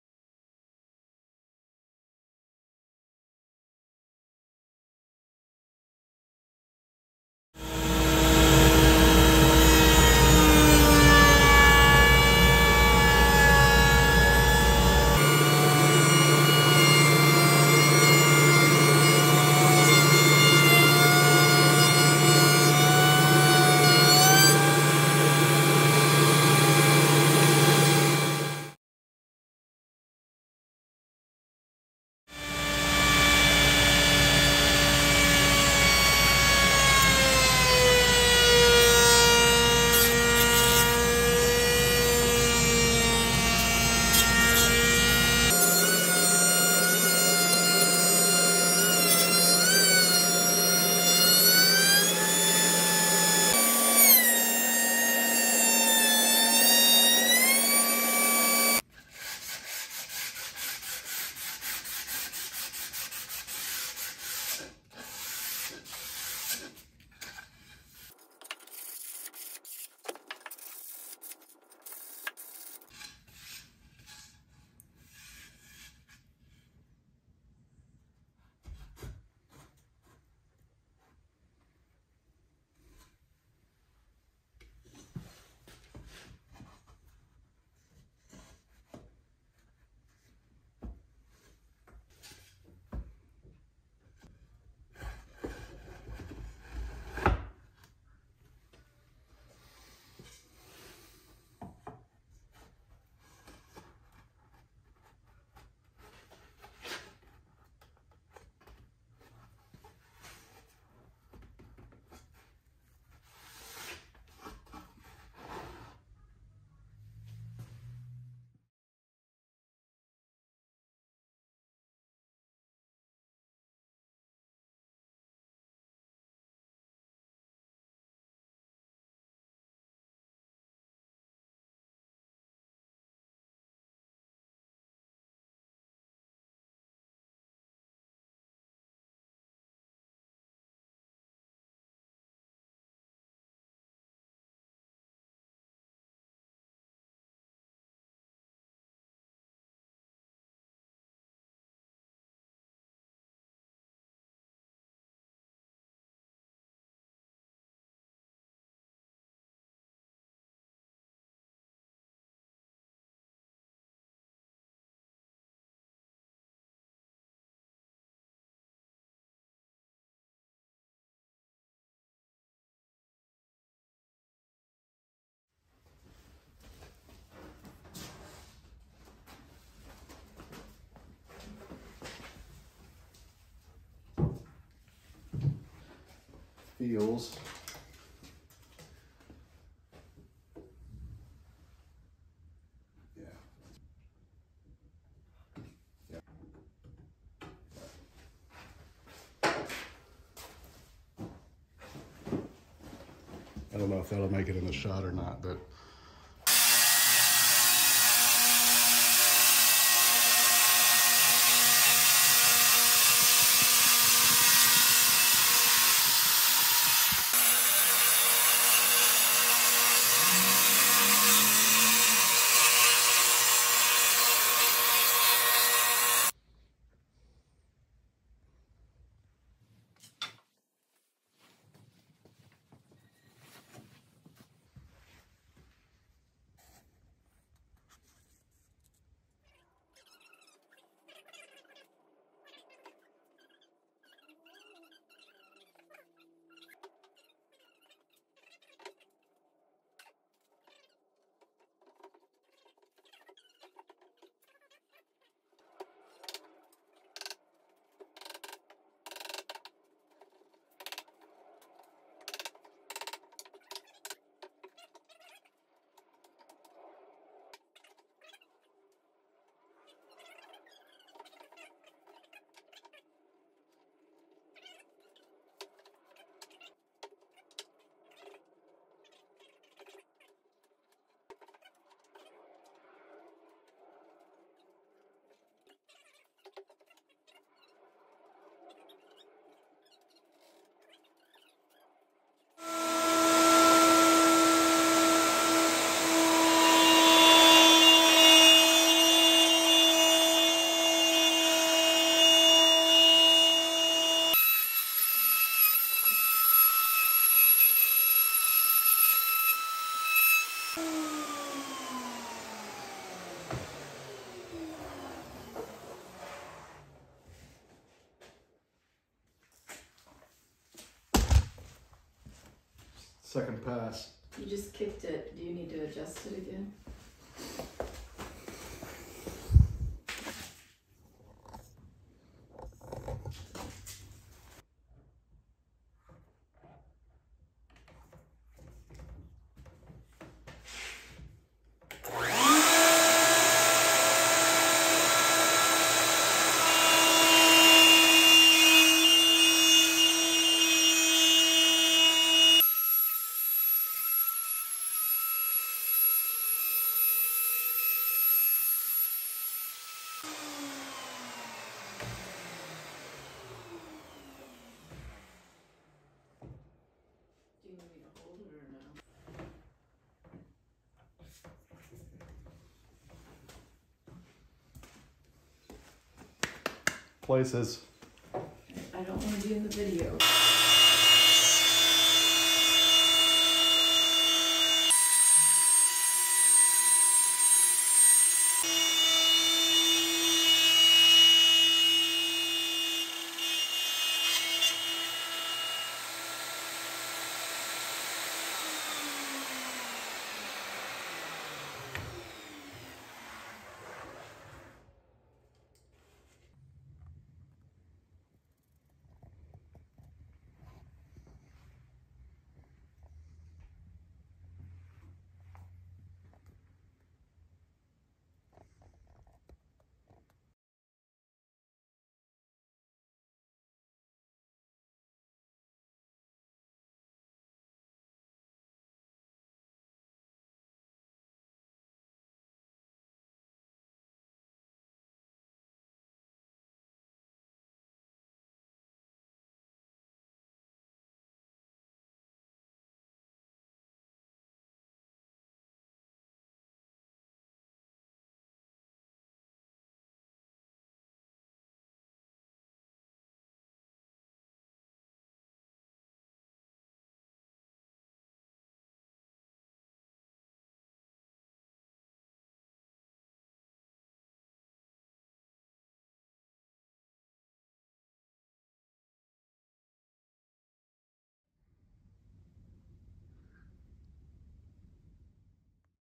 Conference Table Build Process
Conference table in solid cherry I made for a local car dealership about a year ago. This was pre-YouTube channel so I apologize for some missing build steps. 👍👊